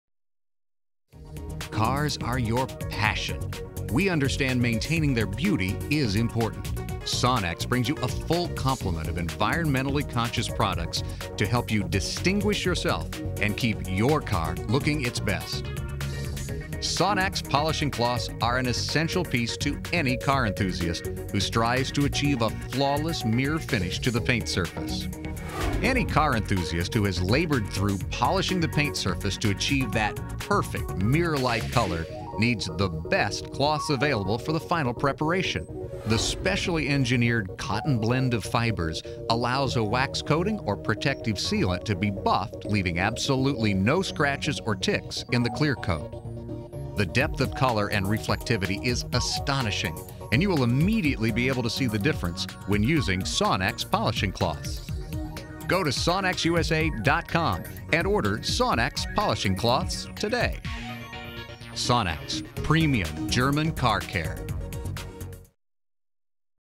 SONAX Polishing Cloths
Specially designed, all cotton cloths for hand polishing or stubborn stained areas.